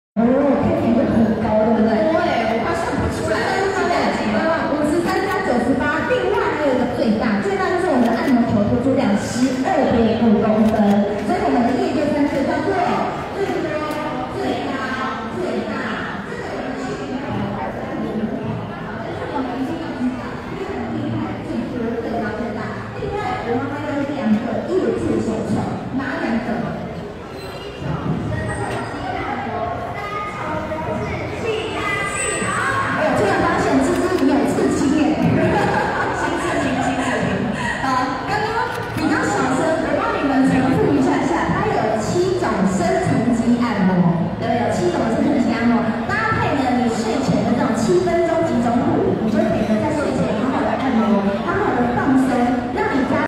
2024.10.05【樂天桃猿】MC 沐妍 & 桃猿男兒(成晉、嚴宏鈞) & 樂天女孩 (曲曲、若潼、穎樂、梔梔) 新竹遠東SOGO百貨《Johnson喬山健康科技》一日店長3
海小弟廟慶民俗、地方人文紀錄站 -【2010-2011年 / 奇摩 Blog 舊照回顧】